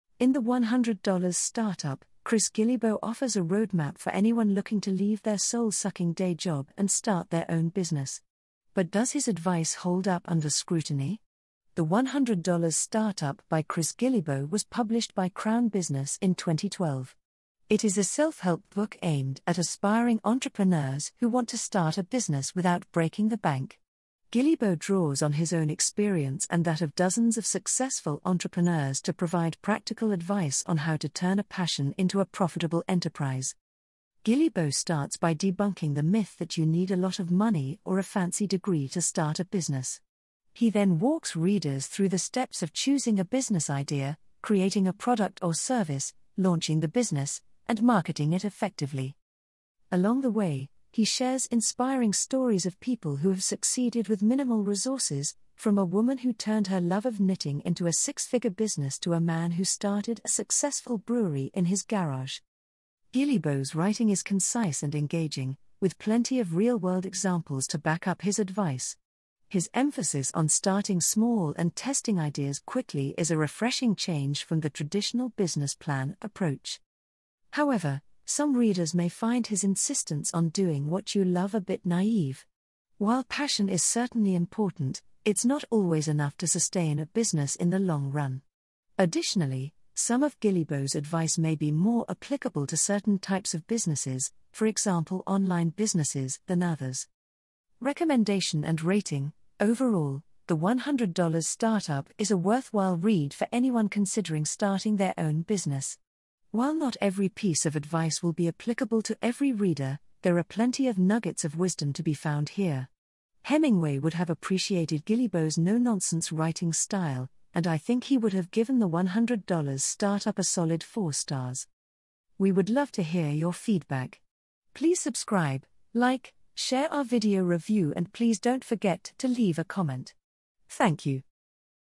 Book Review - The $100 Startup: Fire Your Boss, Do What You Love By Chris Guillebeau
Book Review - The $100 Startup: Fire Your Boss By Chris Guillebeau
Do What You Love and Work Better To Live More

 The $100 Startup: Fire Your Boss, Do What You Love and Work Better To Live More By Chris Guillebeau

Essential Information
"The $100 Startup" by Chris Guillebeau was published by Crown Business in 2012.
It is a self-help book aimed at aspiring entrepreneurs who want to start a business without breaking the bank. Guillebeau draws on his own experience and that of dozens of successful entrepreneurs to provide practical advice on how to turn a passion into a profitable enterprise.

Book Summary
"The $100 Startup" by Chris Guillebeau is a self-help book that provides a roadmap for aspiring entrepreneurs who want to start their own business without breaking the bank.
Guillebeau debunks the myth that you need a lot of money or a fancy degree to start a business and offers practical advice on choosing a business idea, creating a product or service, launching the business, and marketing it effectively.
The book is filled with inspiring stories of people who have succeeded with minimal resources, and emphasizes the importance of starting small and testing ideas quickly.